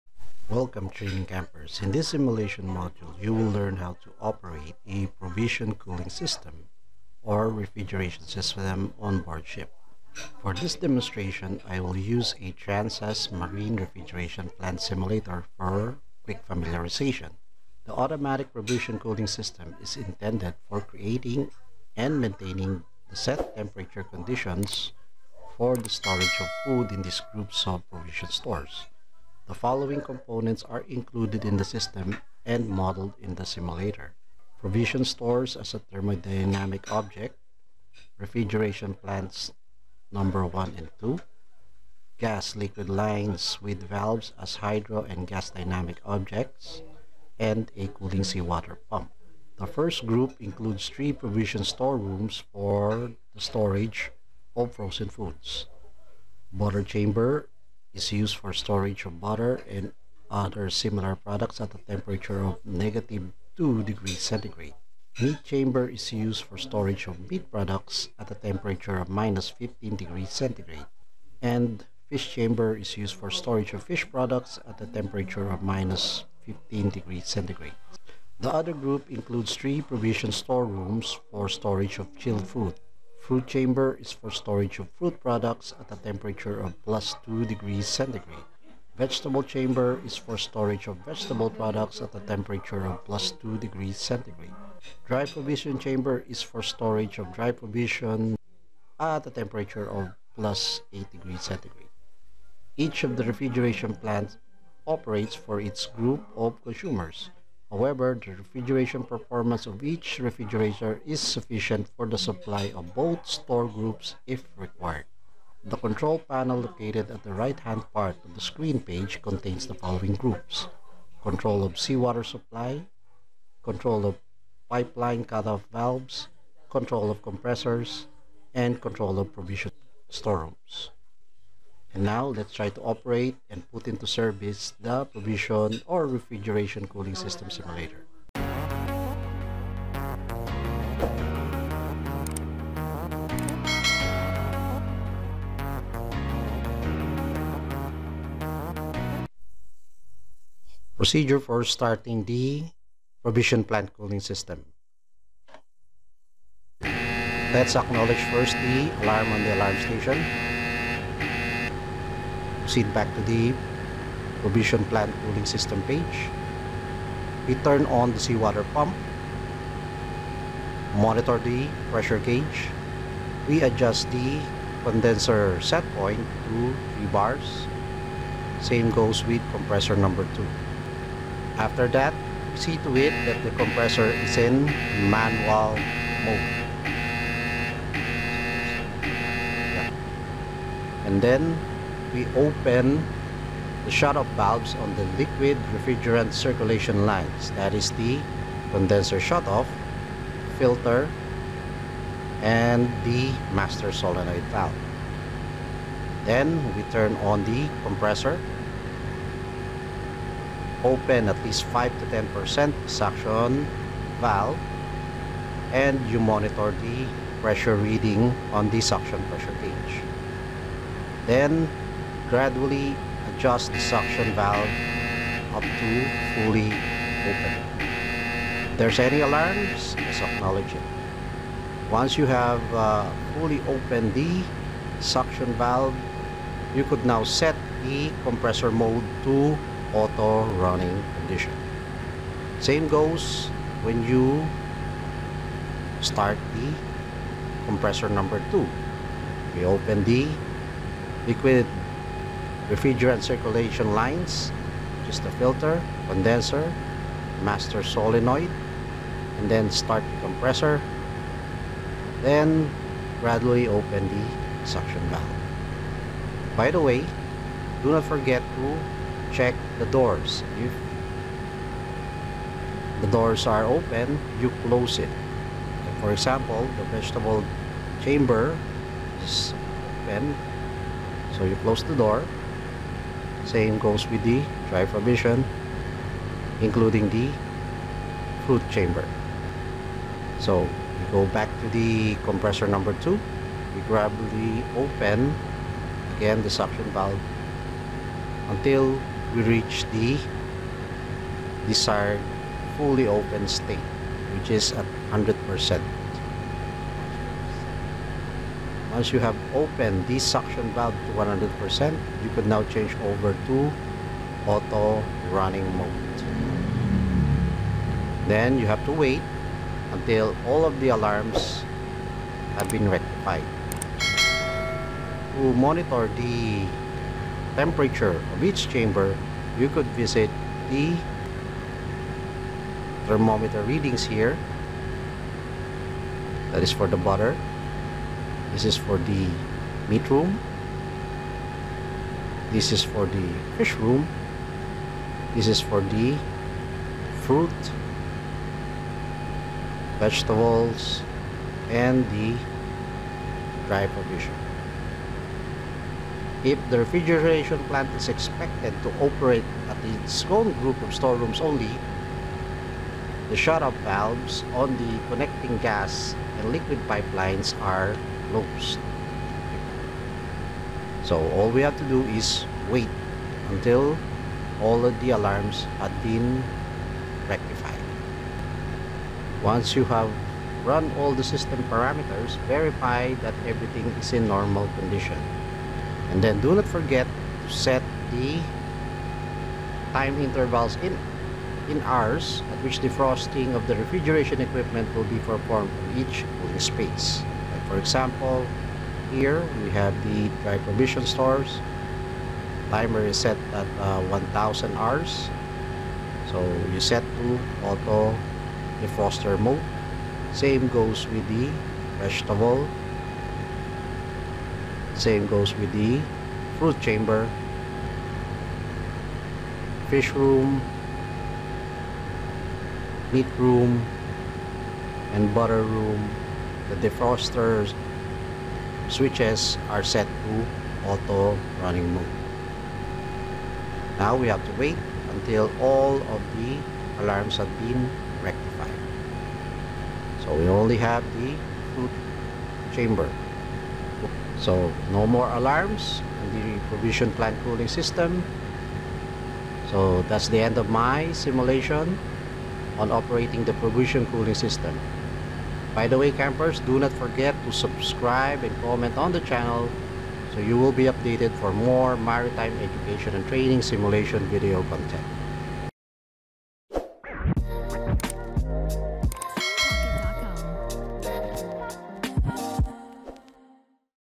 How to Operate a Refrigeration or Provision Cooling System?
00:00 Intro
00:29 System Components
00:55 Store Rooms
01:56 System Control Panel
02:29 Procedures for Starting
04:29 Closing of Store Room Doors
05:29 Monitoring of Store Room Temperatures
06:01 Shut-Off or Isolation Valves
06:28 Setting of Defroster

This simulation clip shows how to operate a refrigeration or provision cooling system which can be encountered onboard ship. It also gives us an idea of the basic parts and controls of the system for easy familiarization and operation.